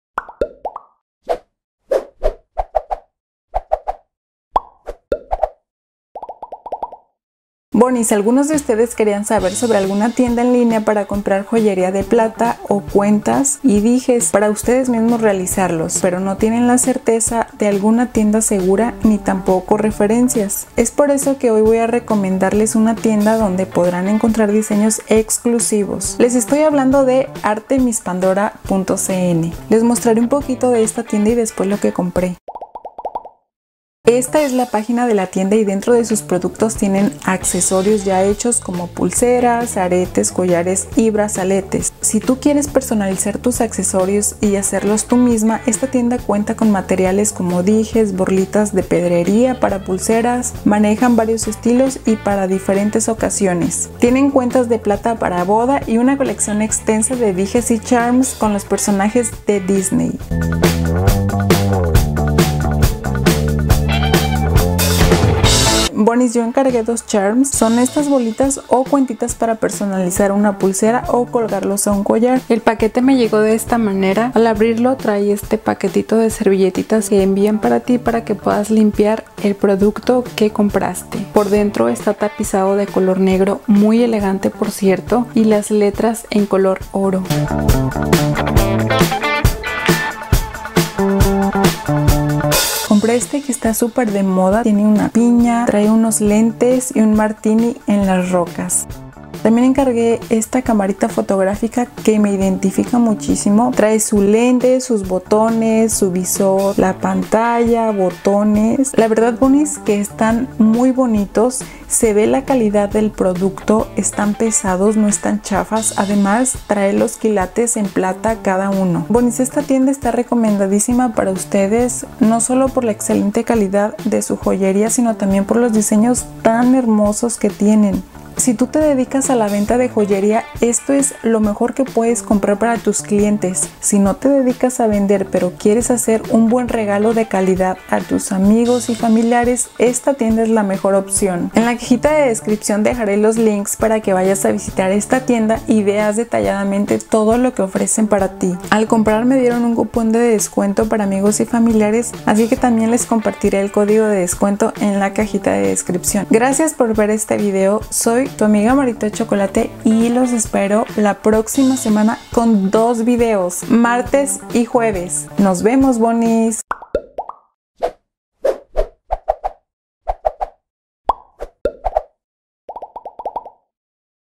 HAUL CAMARA FOTOGRAFICA DE PLATA PANDORA, COMPRA JOYERIA FINA PARA PULSERAS Y DIY COLLARES
BONIS este es un video recomendación y para mostrar lo que he comprado en esta tienda de joyeria fina.
Sus joyas y abalorios son de Plata con diseños esclusivos y personalizados.
Si tú estuviste buscando tanto una camara fotografica de plata para colgarla a una cadena o ponerla en una pulsera o llavero está es la ideal.

Pandora2017

marytochocolate